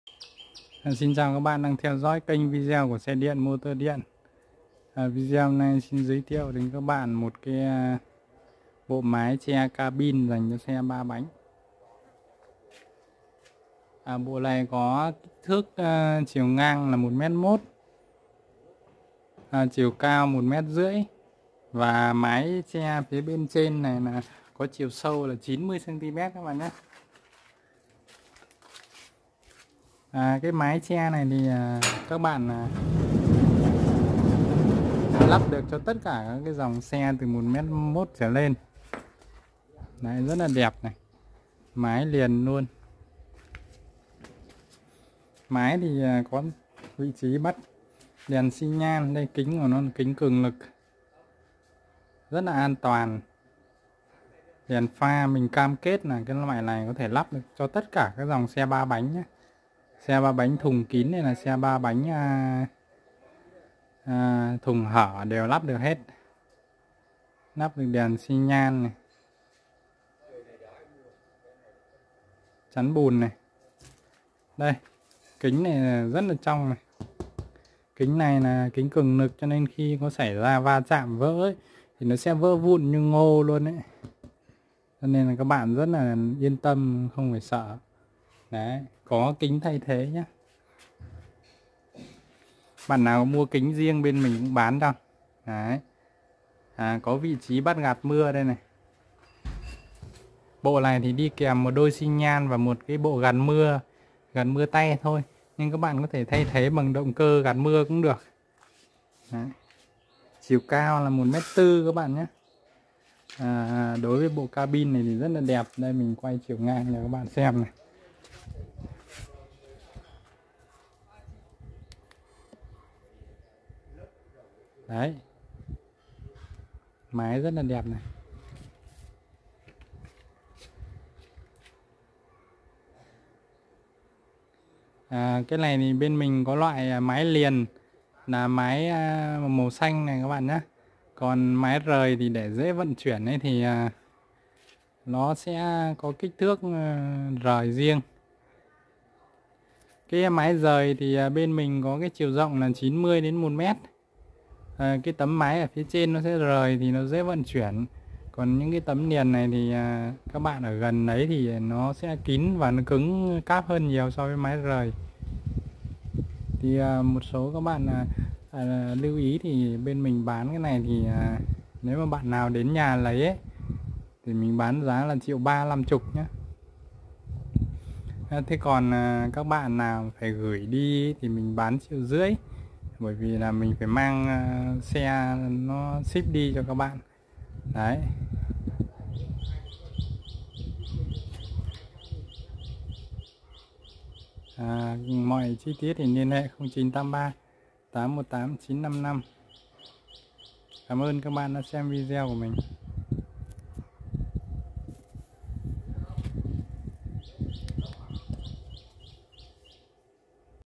Mái che cabin xe 3 bánh tự chế lắp cho tất cả các loại xe 3 bánh GIÁ RẺ CÓ BÁO GIÁ
Mái che cabin xe 3 bánh tự chế lắp cho tất cả các loại xe 3 bánh, giá chỉ 1350k, kính cong cường lực không bị mờ theo thời gian như mika, nhựa... có lưới gạt mưa như ô tô thực thụ, mưa không đến mặt nắng không đến đầu là có thật...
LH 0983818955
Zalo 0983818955